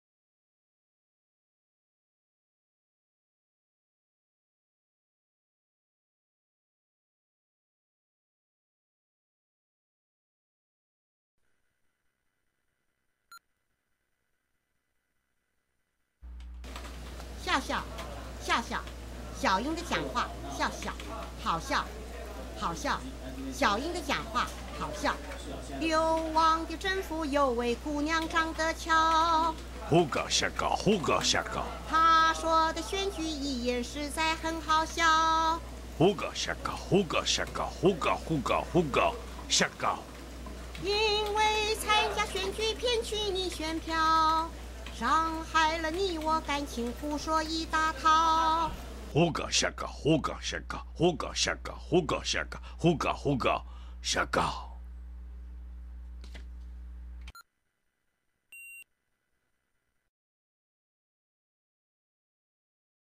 新黨看到的「各說各話」
所謂的一路走來，始終如一，是要有很堅定的信念的。看看過往資料，很多語言，都會因時、地、利不同，而有了不一樣的解讀。就來「哭搜」一下。